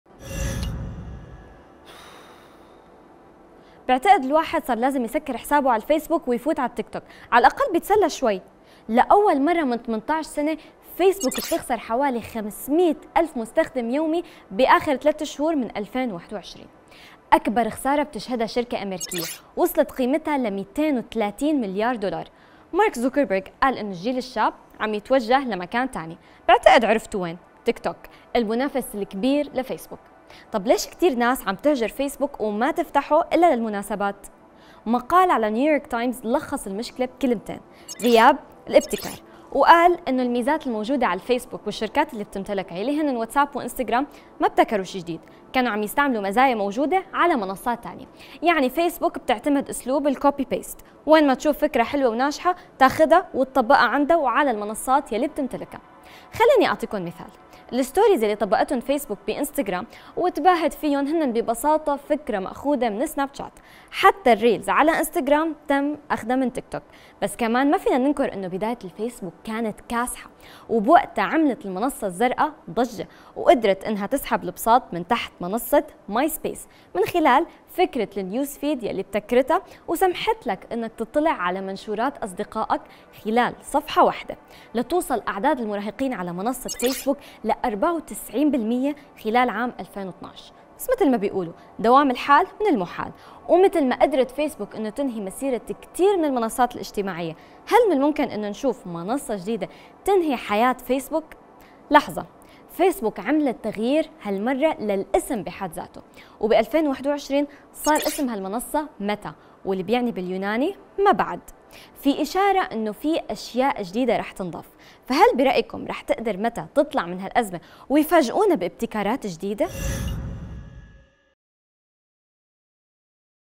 شيء تيك | هل نشهد نهاية مسيرة فيسبوك؟
فيسبوك يفقد أعداد كبيرة من المستخدمين.. ويخسر 230 مليار دولار.. والسبب في خطأ تمارسه المنصة منذ فترة طويلة.. هل نهاية فيسبوك قريبة؟